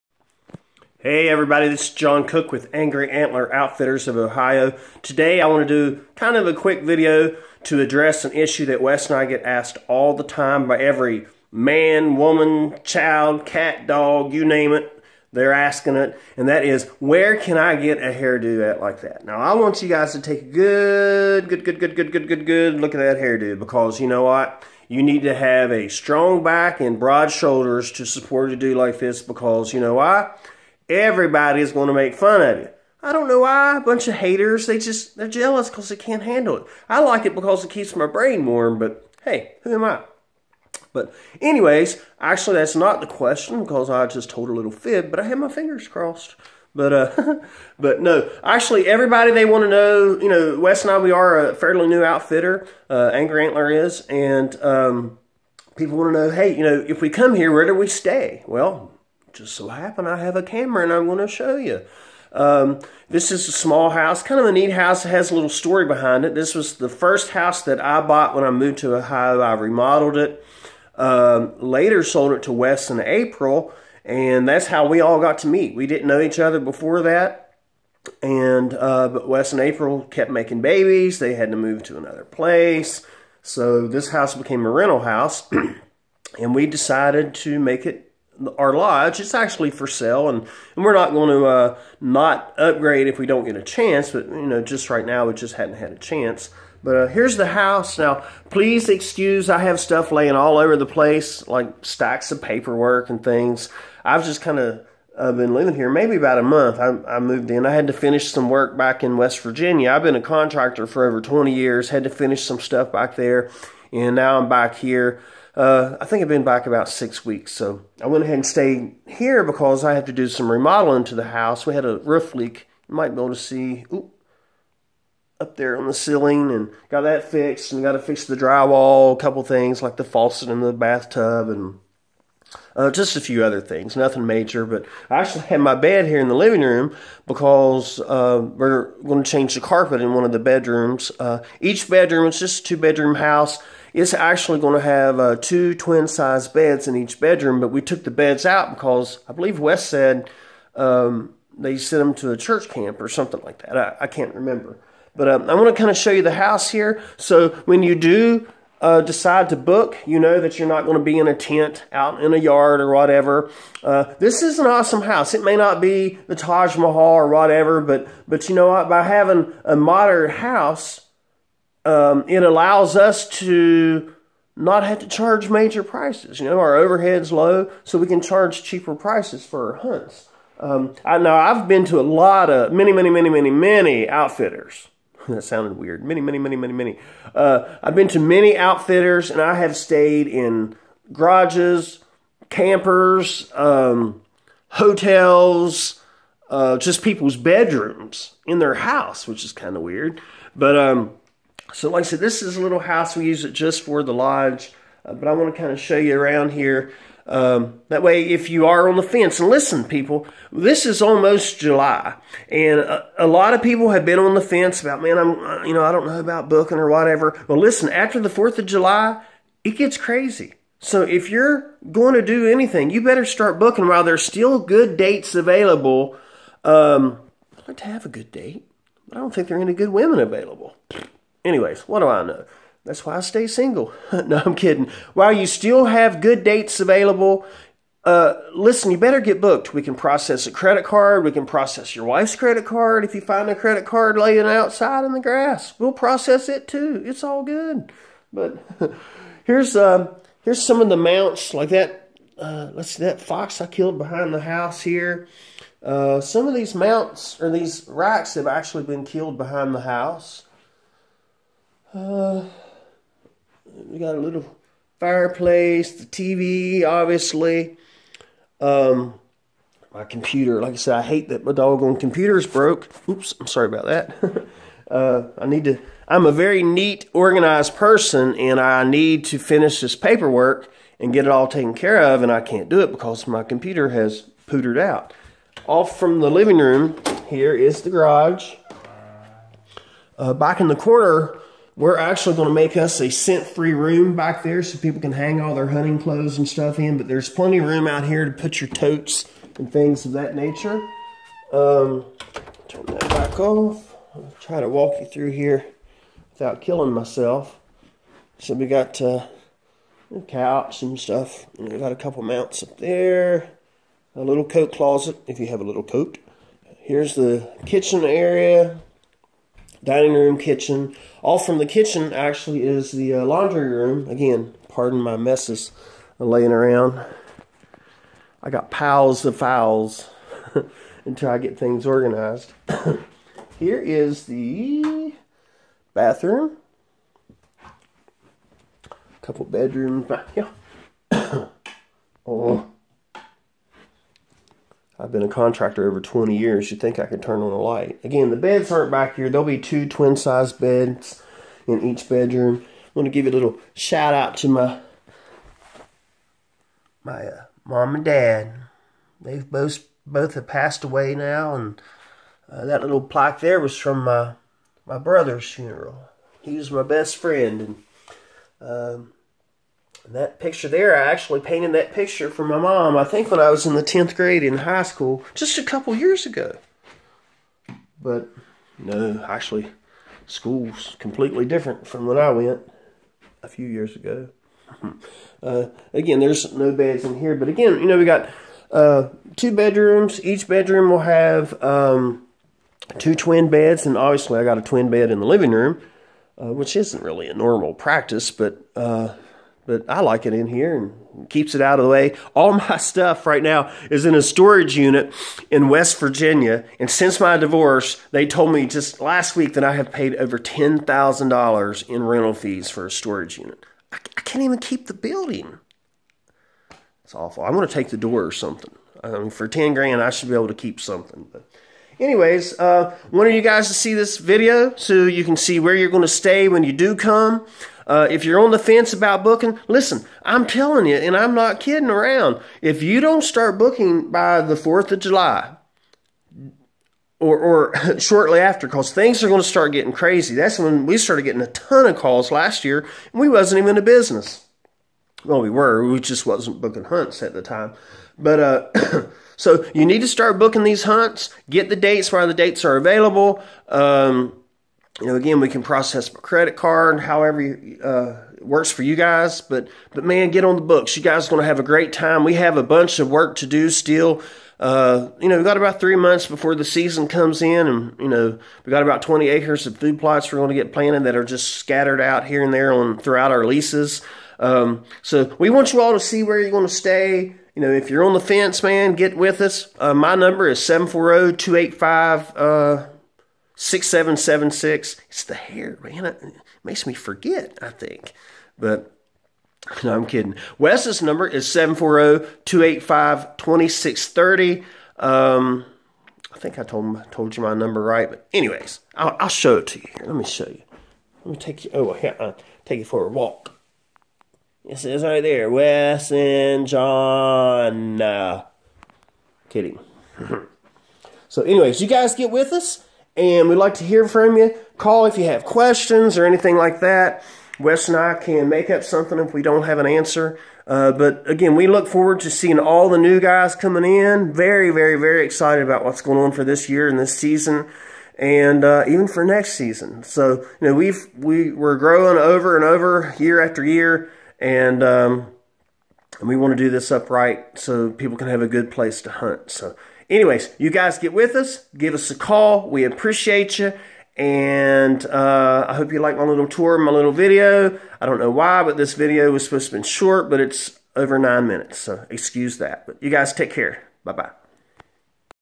Crib/lodge tour for Angry Antler Outfitters of Ohio
Just a simple walk through to so folks they won't be staying in a tent. unless they want to :)